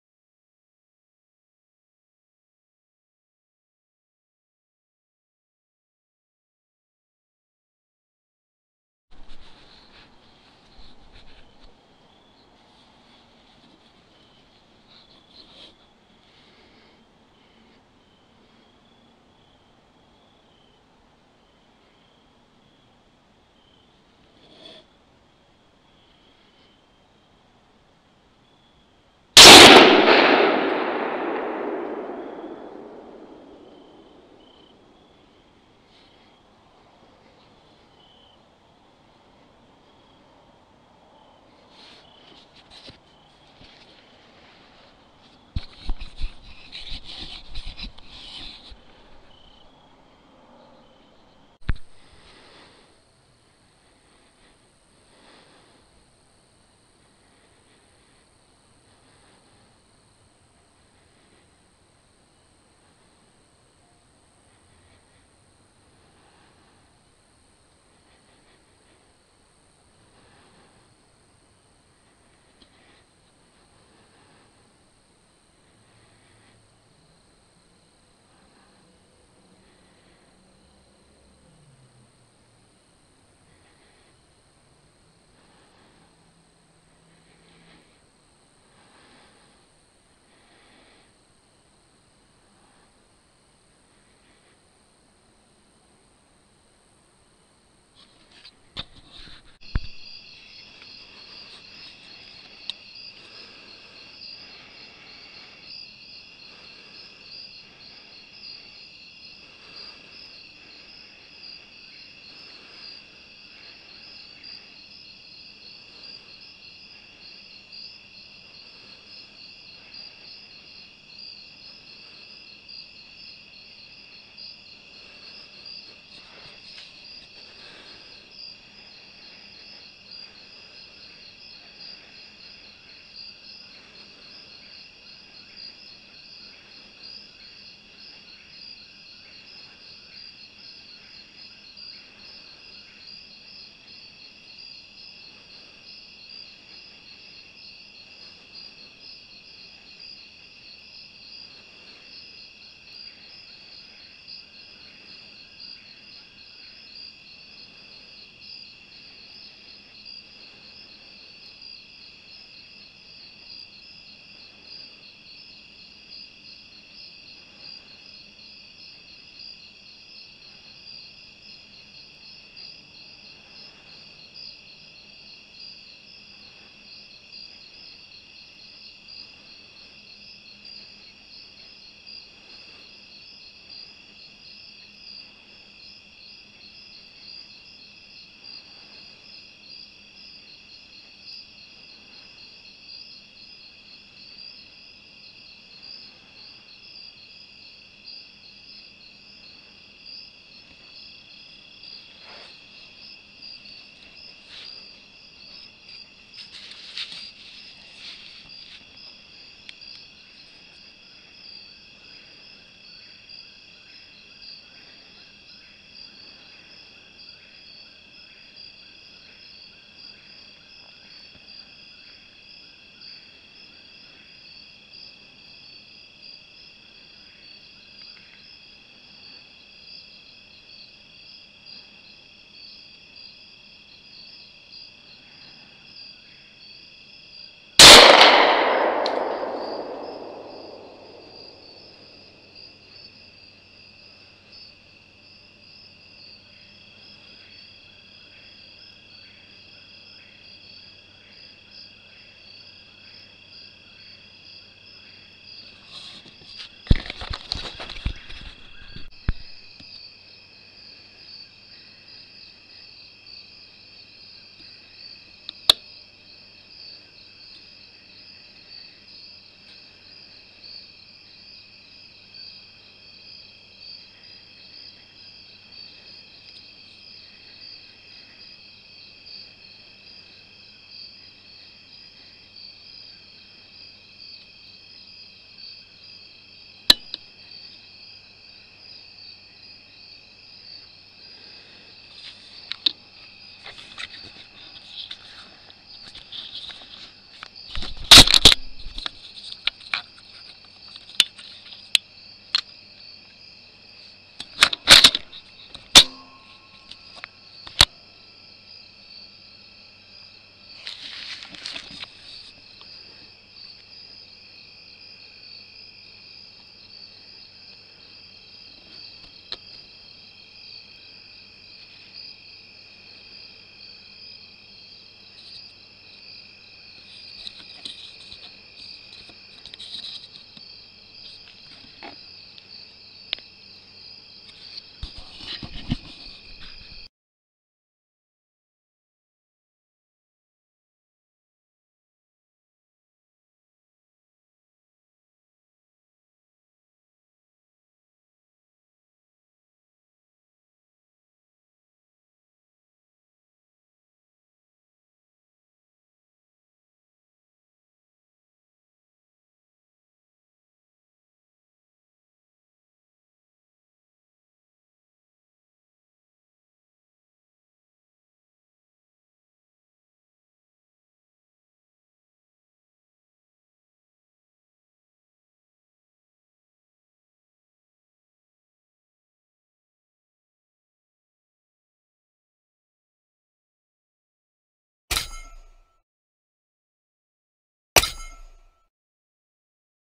SightMark Wraith in action Hog Hunting at night in South Carolina
My Friend David took these videos with his Sight Mark Wraith Mini . It can change color of the reticle and you can change the type of reticles in the scopes menu David perfers the circle dot . He missed a few pigs in these videos , He was perched in a two man ladder stand 21 feet in the air that feeder is 90 yards from the stand and the shooting rail is not very stout .